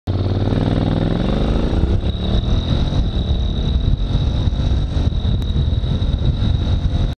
zero to 60
Standing start to 100 kph in 7 seconds. It's possible I could do it faster with manual shifting. I'm in S2 mode here.